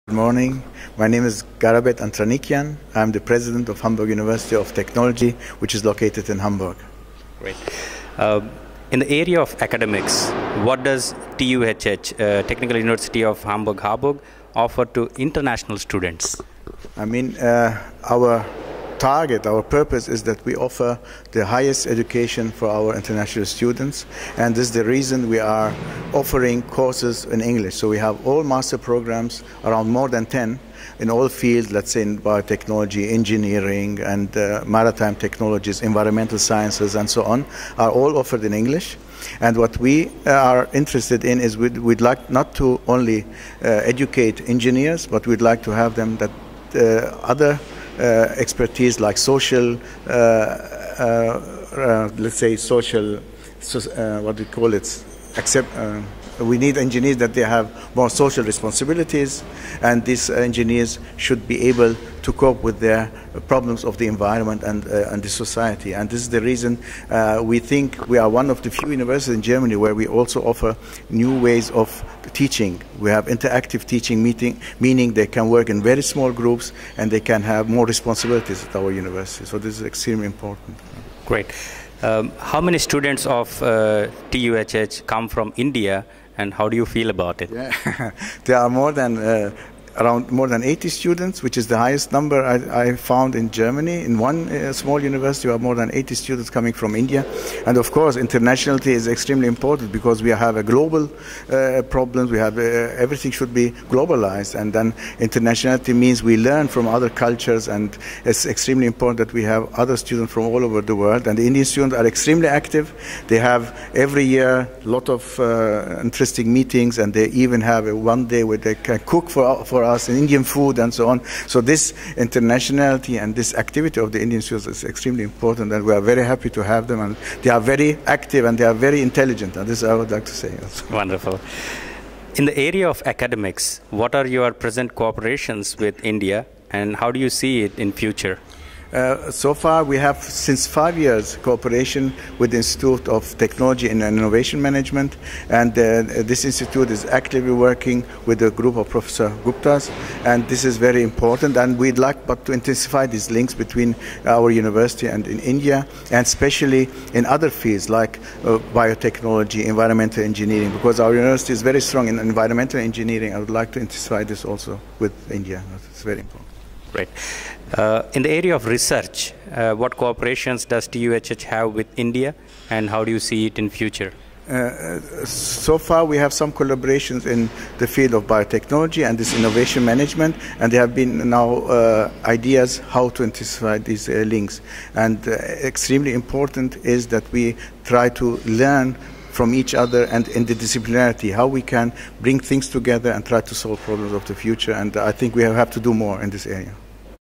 With the President of TUHH
Hamburg Oct. 2011 - Prof. Dr. Garabed Antranikian, the president of Technical University of Hamburg Harburg sharing his views with Mr. Gopi V. Prasad, the Managing Director of GloMan group.